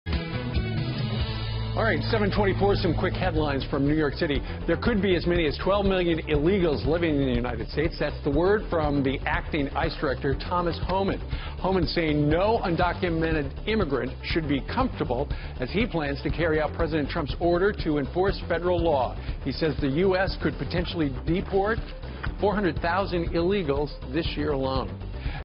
Acting ICE director: 11-12 million illegals in the US
Agency plans to arrest and deport 400 thousand this year